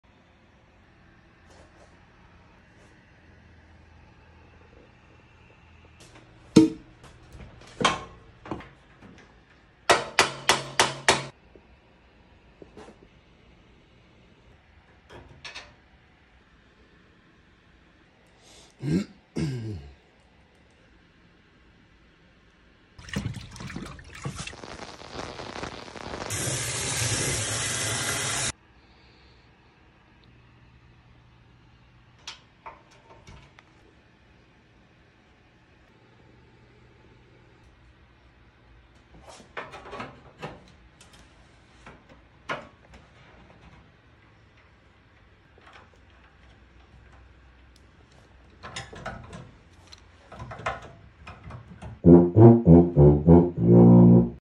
Tuba Spa Day- band instrument repair- Wes Lee Music Repair
Tuba gets chemical flushed, uniball repairs, new bumpers, and just overall good playing condition repairs. We call this the “spa treatment “.